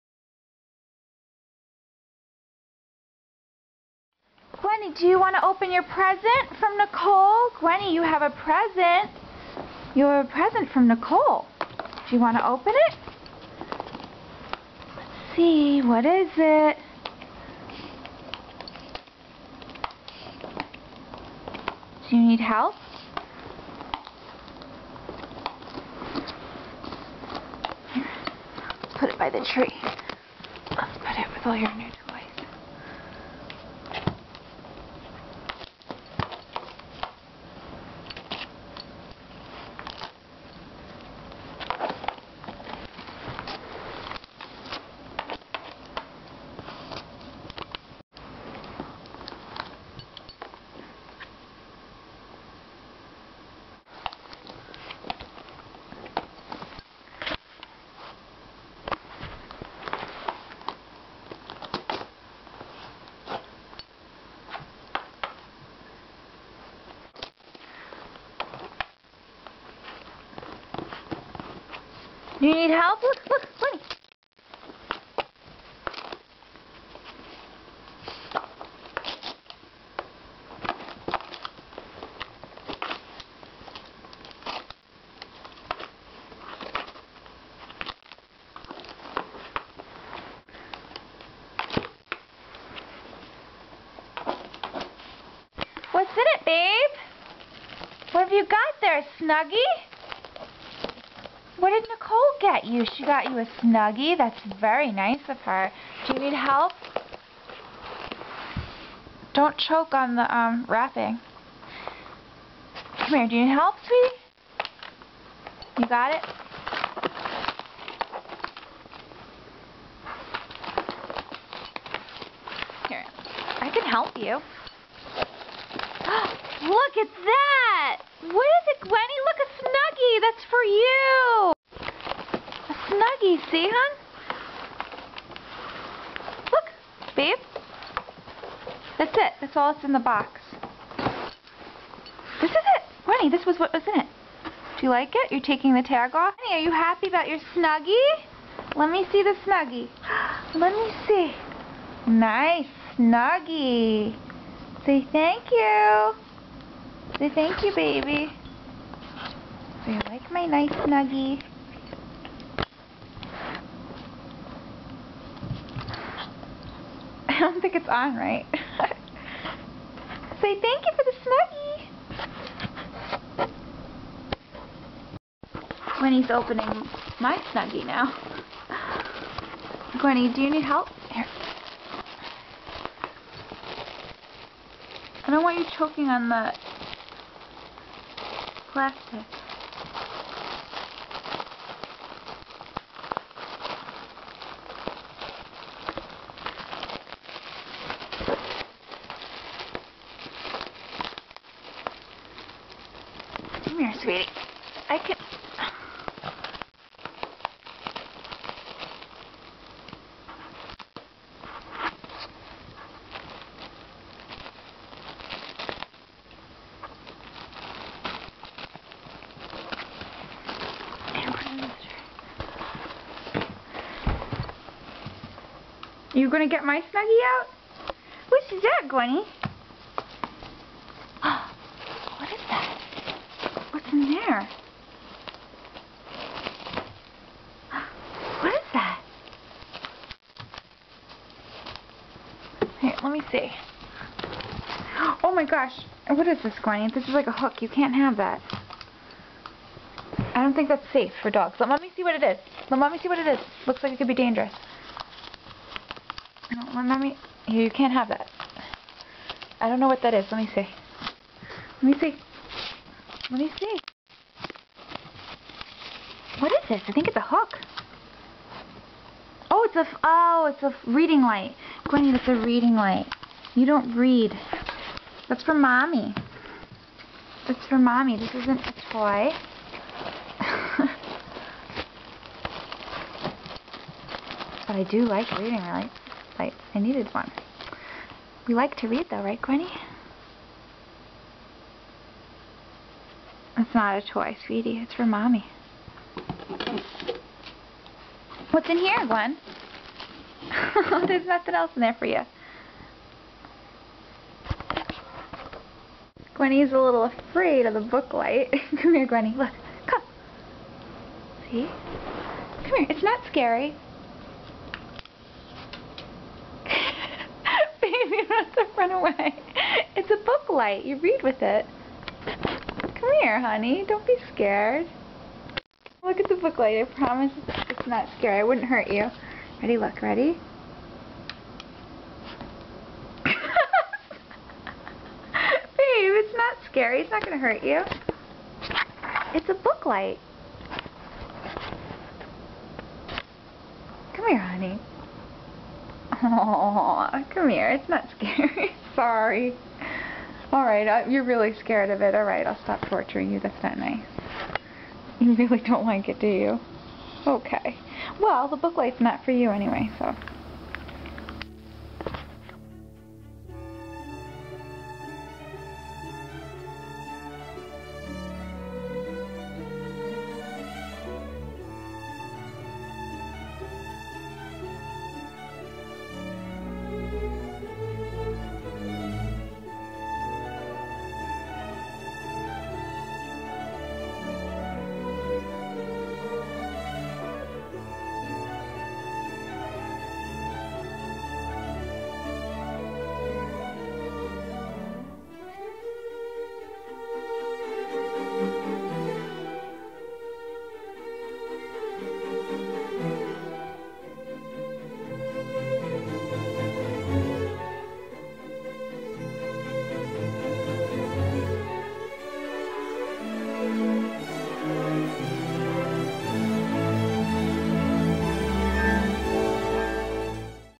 Adorable Bichon unwraps a Snuggie!! She is so excited!!!
My dog Gwenny unwrapping a Snuggie that she got for Christmas.